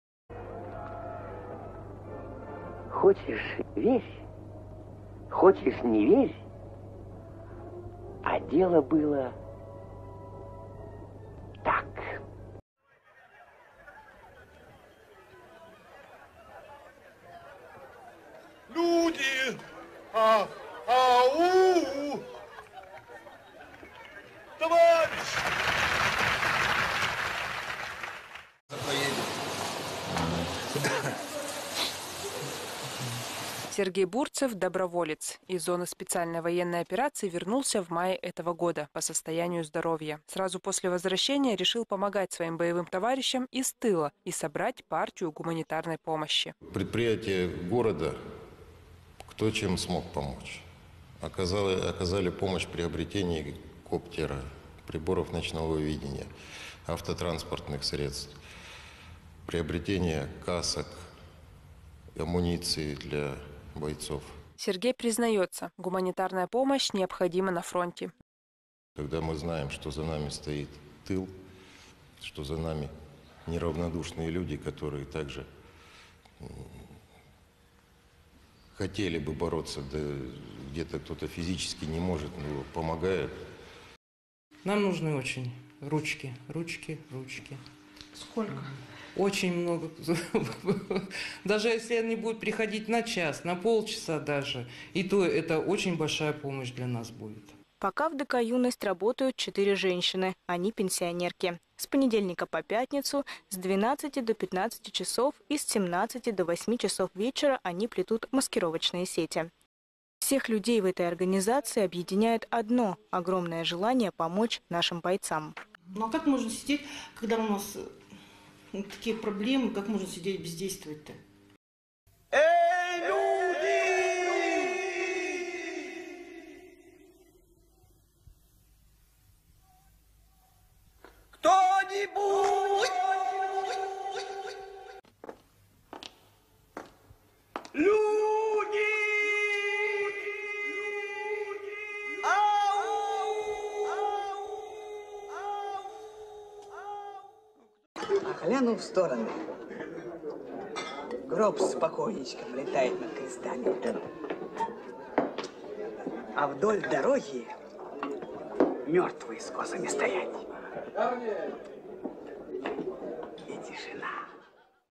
ИЩУТ ПОЖАРНЫЕ, ИЩЕТ МИЛИЦИЯ...
.Организаторов всенародного плетения маскировочных сетей для нужд наших витязей скоро кондратий схватит от бесконечной саморекламы.